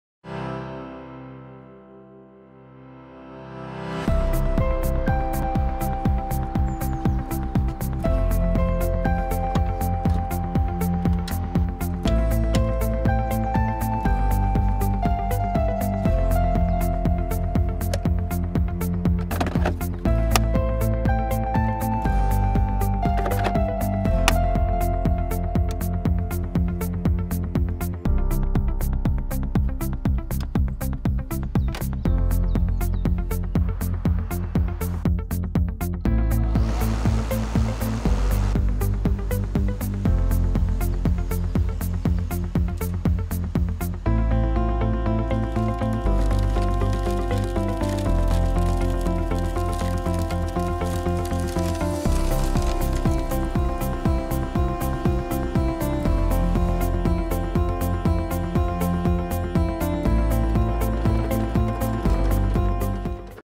Husqvarna LC 247i - Mașină de tuns gazon, cu acumulatori
Mașina de tuns gazon LC 247i din gama Hsusqvarna cu acumulatori este proiectată pentru zone rezidențiale, unde noxele și zgomotul sunt surse de poluare nedorite de locuitori. Reglarea centralizată a înălțimii de tăiere vă ajută să coborâți sau să urcați rapid puntea de tăiere la înălțimea dorită. Sistemul unic de acumulatori și compartimentul dublu pentru utilizarea a două baerii vă oferă autonomie extinsă. Funcție save pentru consum și zgomot reduse. Start intuitiv și comenzi la îndemână. Greutate redusă. Power boost automat pentru putere atunci când ai nevoie.